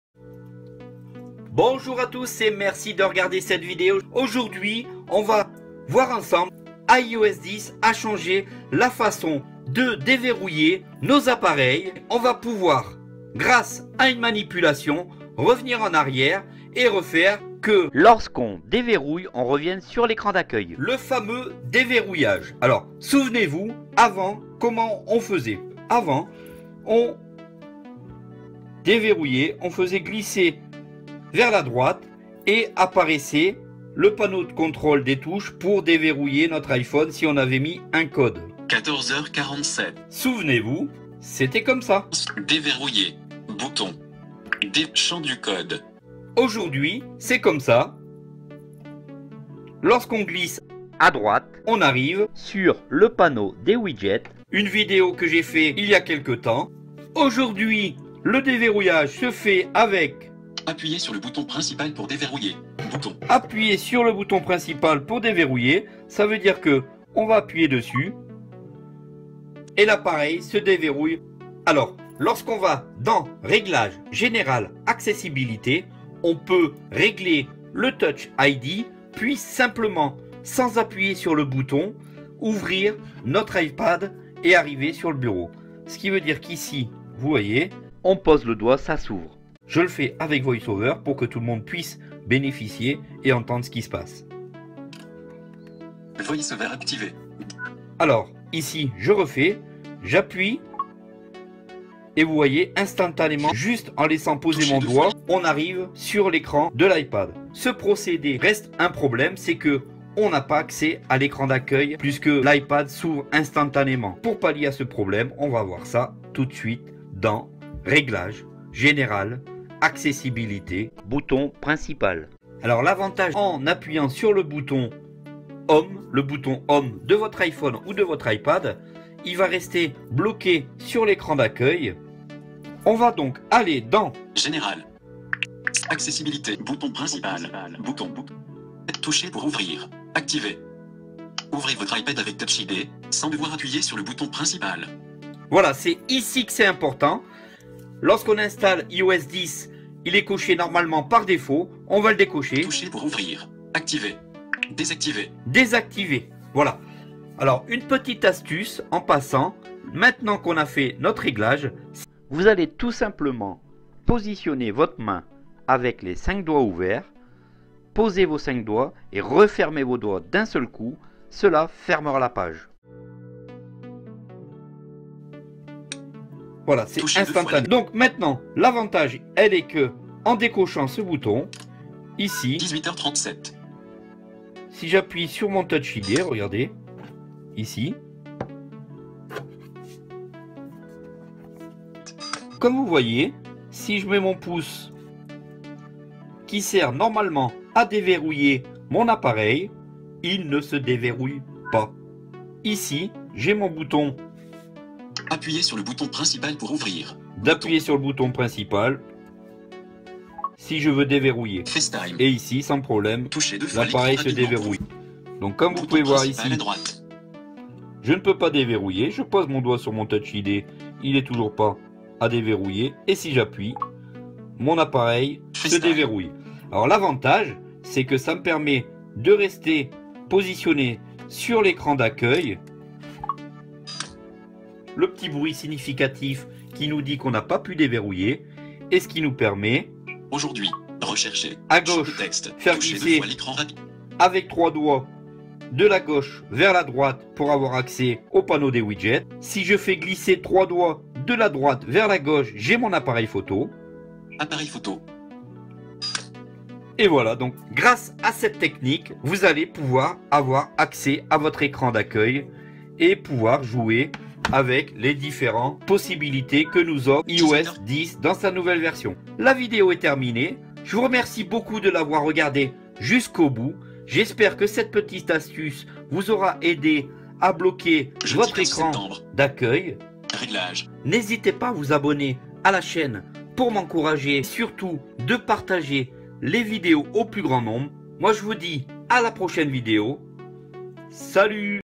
DÉVEROUILLER TOUCH ID COMMENT MODIFIER [L'OUVERTURE DE VOTRE IPHONE]
Salut à tous iOS 10 à intégré une nouvelle façon de déverrouiller votre iPhone iPad iPod touch mais parfois on à pas le temps de rester sur l'écran d'accueil petite astuce qui va permettre de rester sur l'écran et pouvoir aller sur les widgets ainsi que sur appareil photo à parti de l'écran d'accueil si la vidéo vous plaît abonné vous cela me fera plaisir et m'encouragera merci à vous .